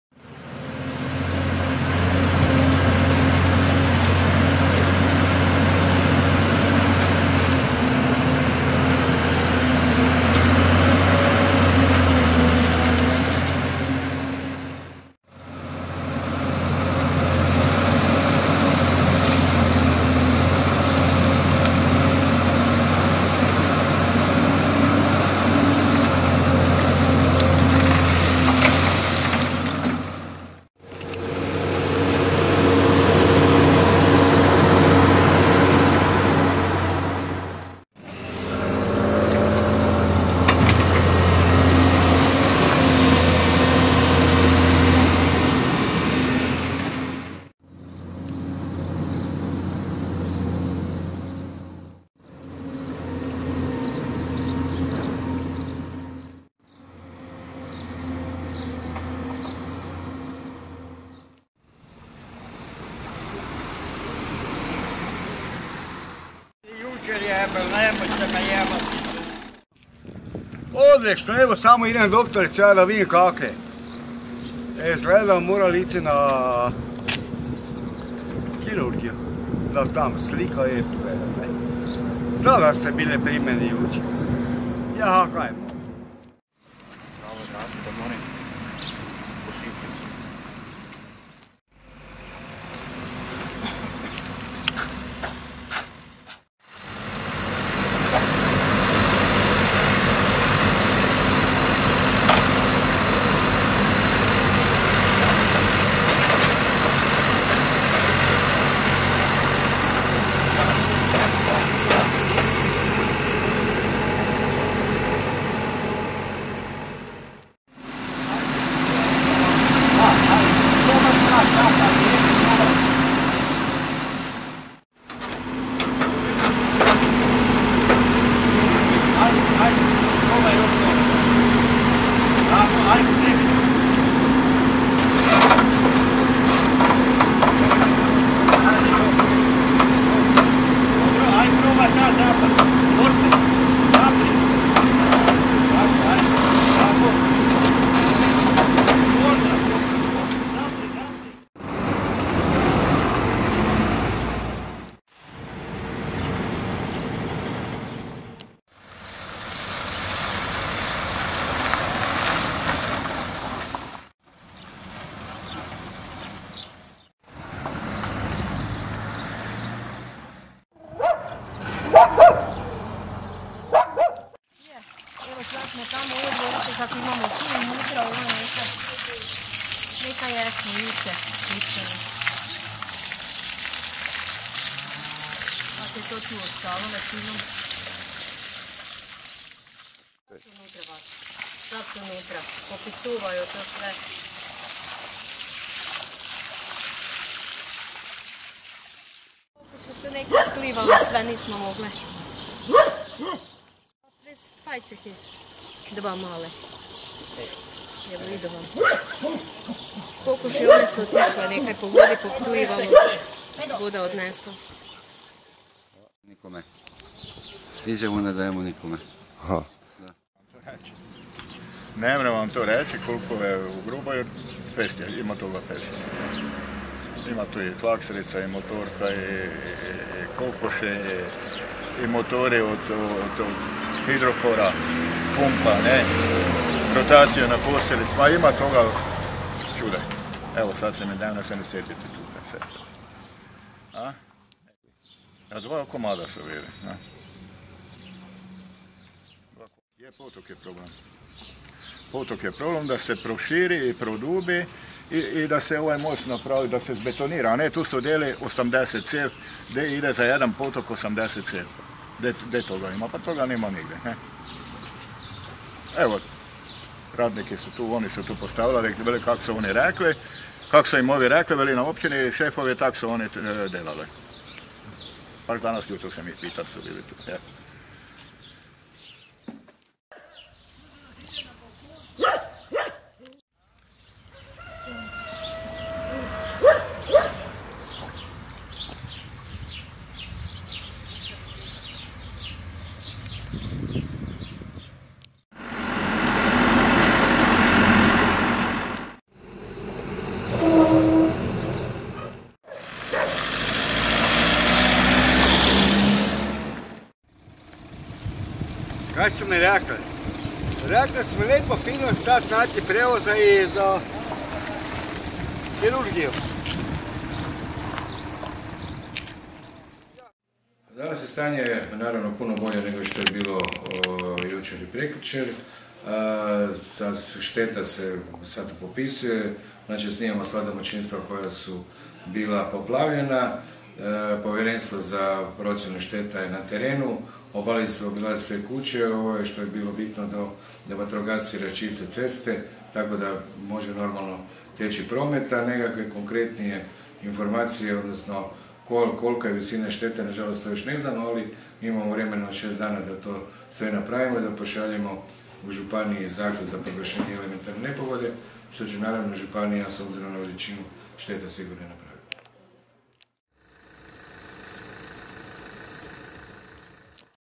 Sanacija štete u Rasinji nakon poplava
Iako se drugi dan od velikih vodenih bujica koje su preplavile cijelo mjesto život polakao vrača u noramlu, situacija u Rasinji je još uvijek daleko od idealne, jer tragovi razaranja vode i velike štete vidljive su na svakom koraku. Sanacija stanja je u tijeku, a danas je na teren izašla i komisija za procjenu šteta koja ce popisivati stanje sljedećih dana s ciljem da župan proglasi elementarnu nepogodu.